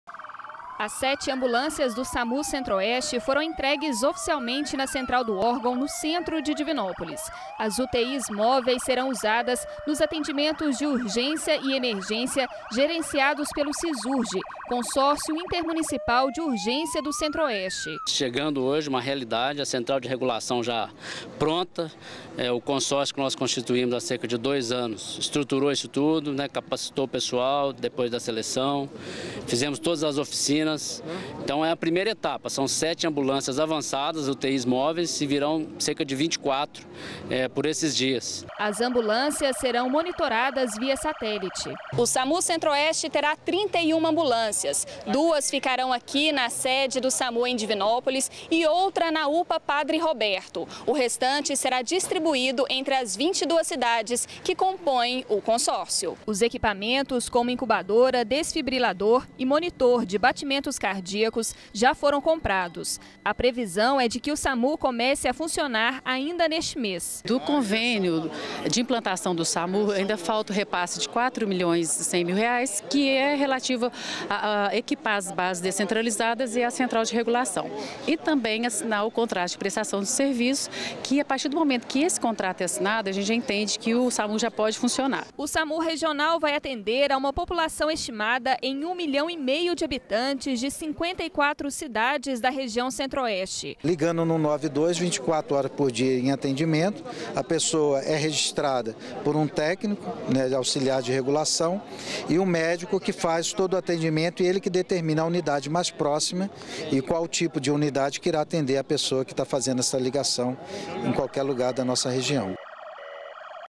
Sete UTI's móveis são entregues para o Samu Centro-Oeste
Para o Samu começar a funcionar, falta a liberação de uma verba de R$ 4 milhões. A prefeitura acredita que os atendimentos vão começar ainda neste mês.